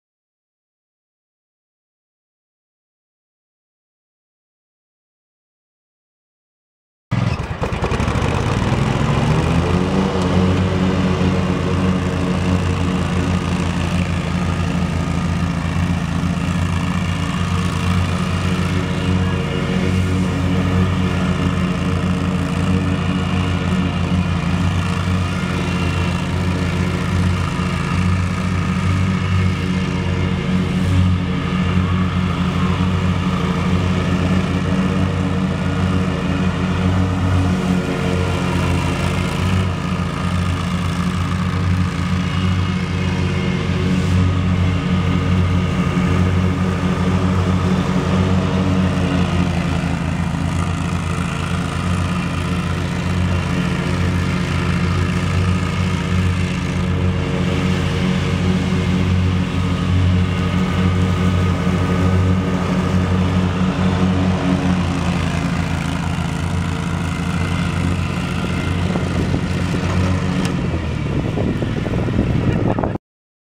2006 Hustler Super Z lawn mower for sale | sold at auction March 19, 2014
2006 Hustler Super Z lawn mower
2,809 hours on meter
Model 928036
72" deck
Kohler Power 30 HP engine
Hydrostatic drive
Custom bucket mount
Serial 06060881

It was located in Fredonia, KS, and we sold it to the highest bidder for $2,750.